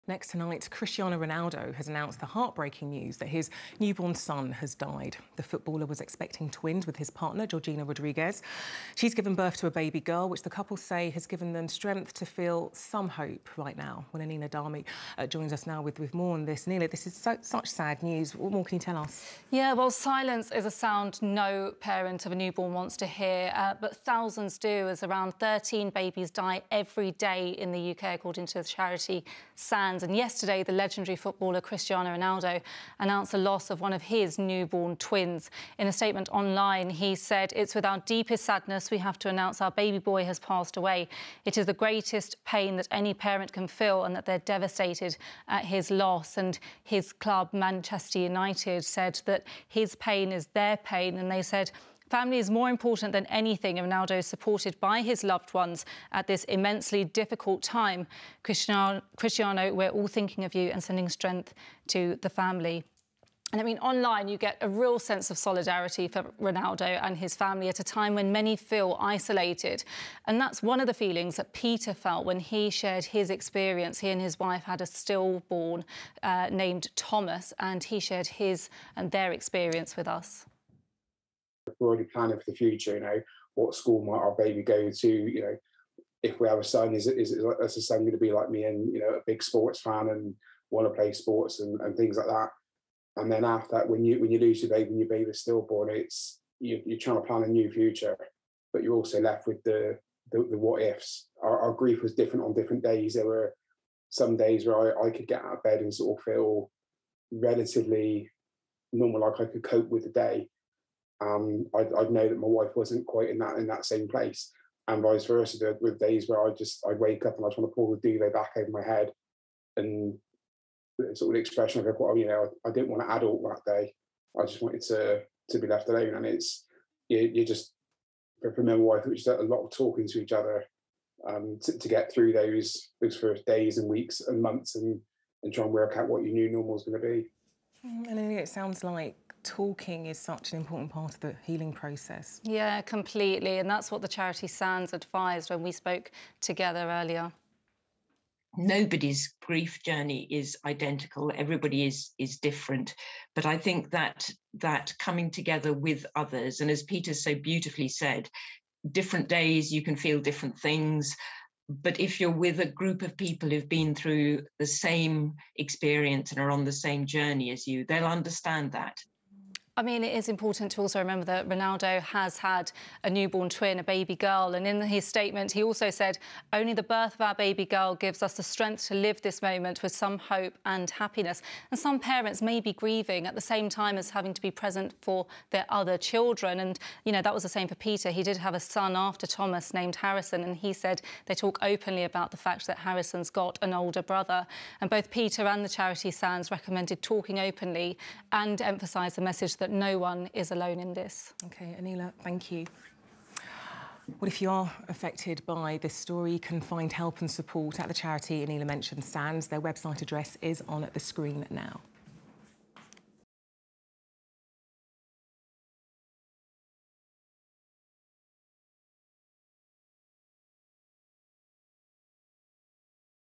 Cristiano Ronaldo and partner Georgina Rodriguez heartbroken over the loss of their baby boy| 5 News
►Cristiano Ronaldo has announced the heart-breaking news that his new-born son has died. The footballer was expecting twins with his partner Georgina Rodriguez. She has given birth to a baby girl, which the couple say has given them strength to feel some hope right now.